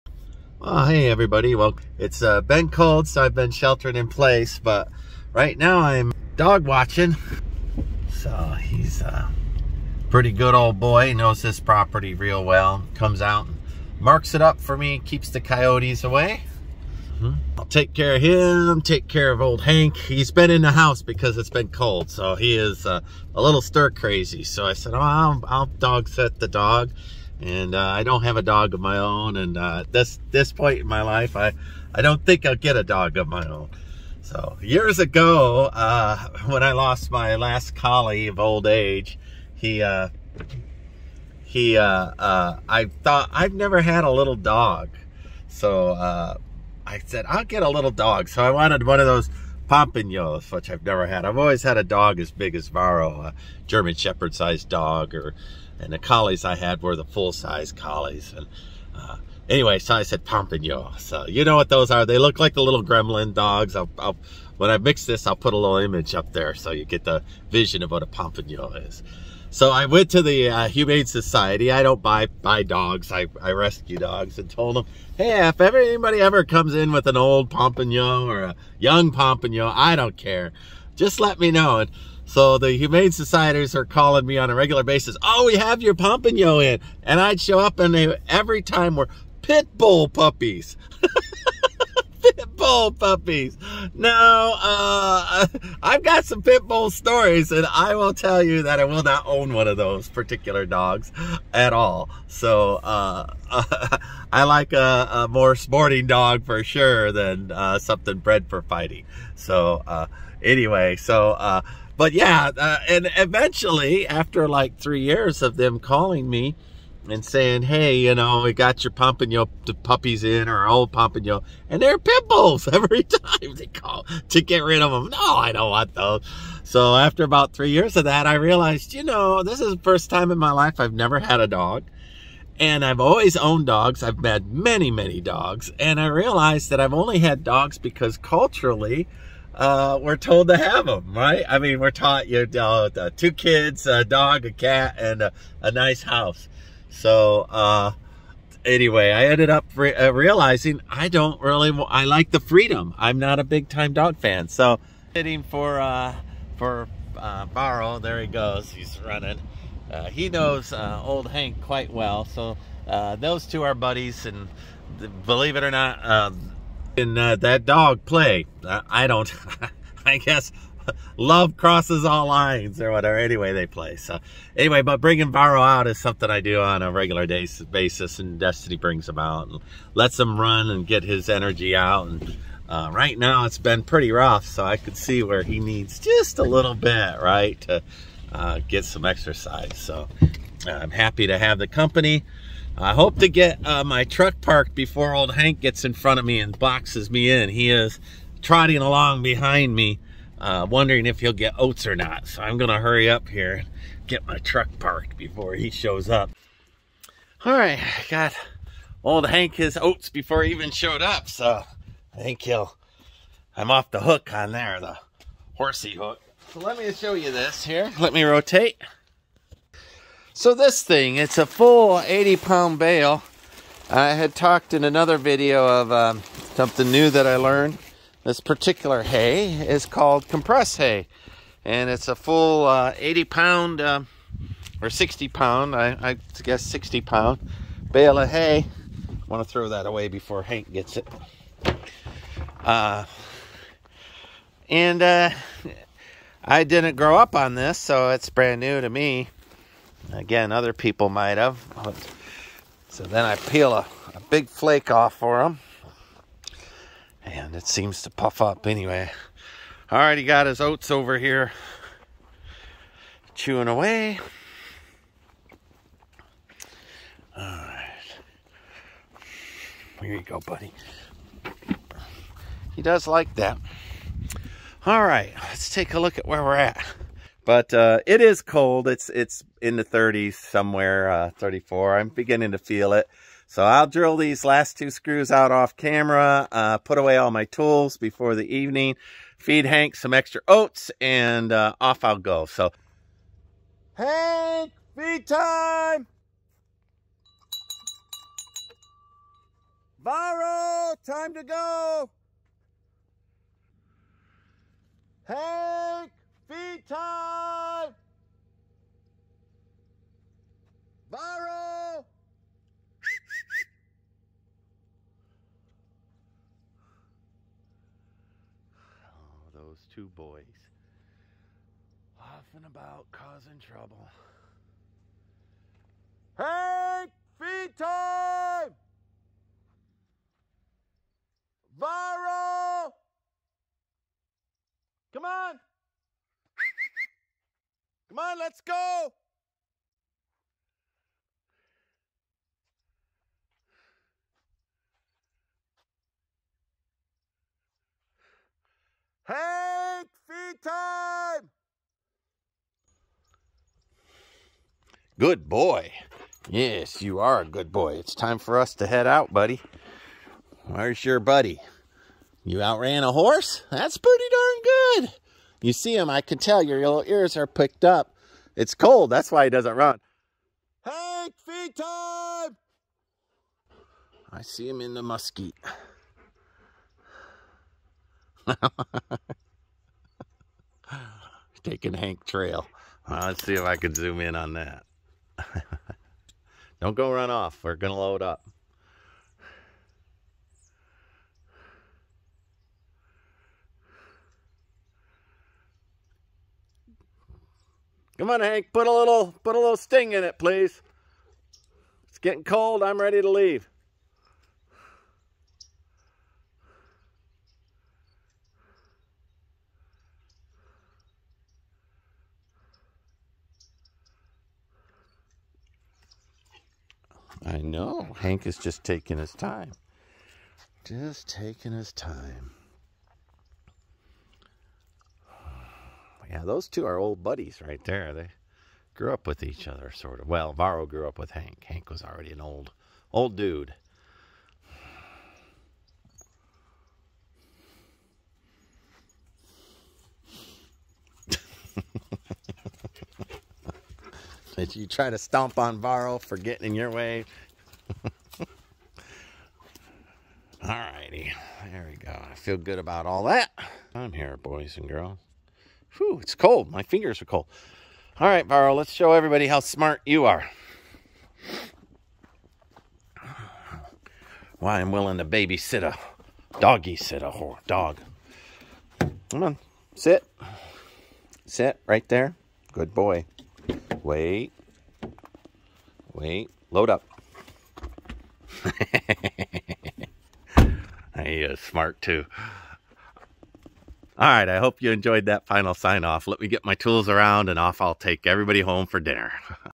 Wolf dog and old Hank the Horse check-in during the deep southern freeze on the 1000 Year Ranch
I am checking in on the animals during the deep southern freeze. Sheltering in place means the wolf gets tired of the house and needs to run. Old Hank the Horse needs fed and looked after.

It seems like everything is in good order and the wolf and horse are excited for the day of fun in the cold northern vortex air.